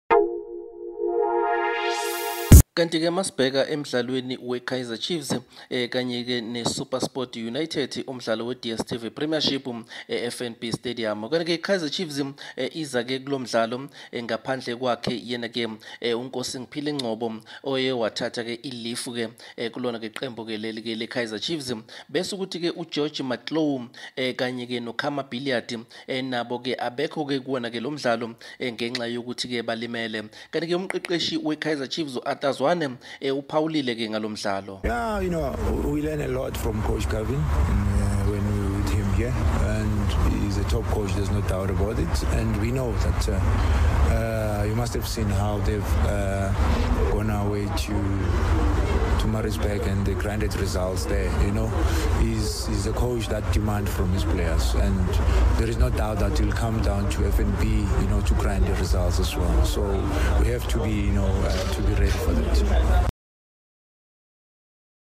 Kaizer Chiefs vs Supersport United at FNB Stadium
Amakhosi 4 life will hosts Supersport United under Gavin Hunt in the DStv premiership match at FNB Stadium. The glamour boys will be without Nkosingiphile Ngcobo out on leave , George Matlou and Khama Billiat both injured . Coach Arthur Zwane shared his views .